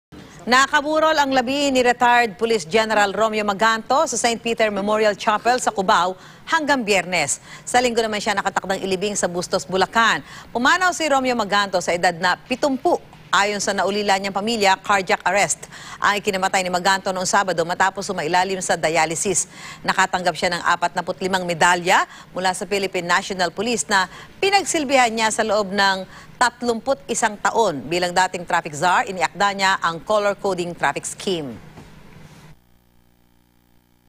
UB: Ret. Police Gen. Romeo Maganto, pumanaw sa edad na 70; nakatakdang ilibing sa Linggo
Nakaburol ang labi ni retired Police General Romeo Maganto sa St. Peter Memorial Chapel sa Cubao hanggang Biyernes.